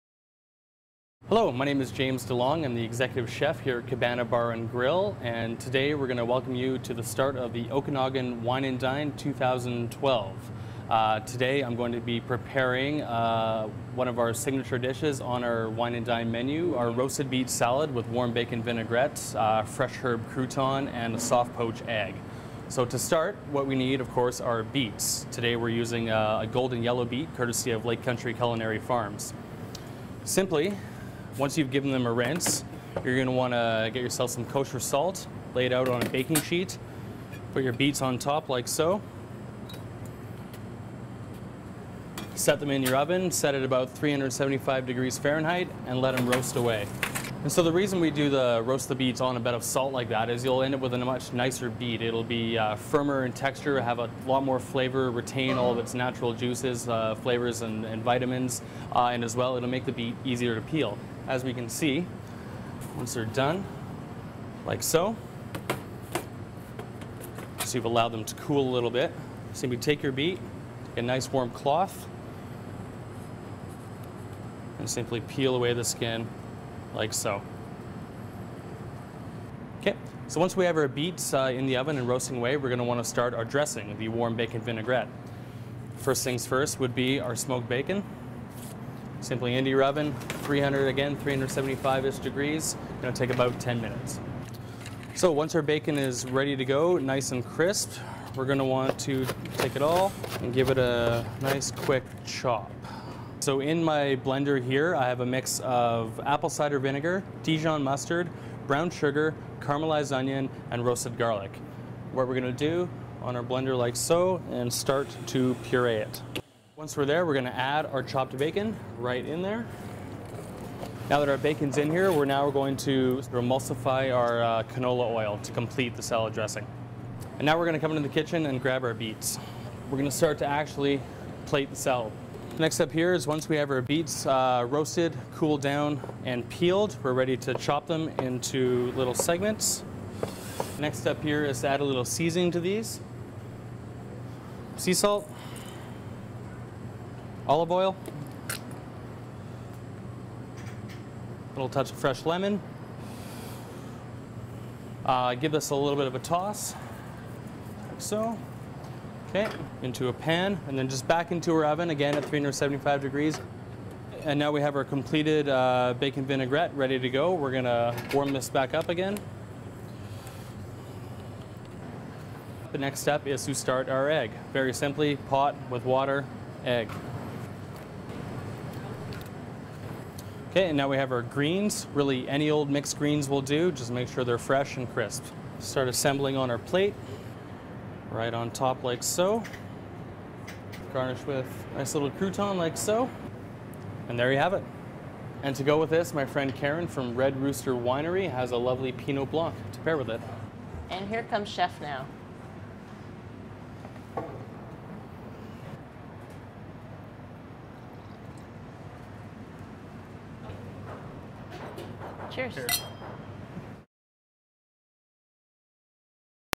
Wine and Dine with Cabana
Executive Chef James DeLong, of Cabana, shows us how to create a warm beet salad with a bacon vinaigrette to pair with Red Rooster's Pinot Blanc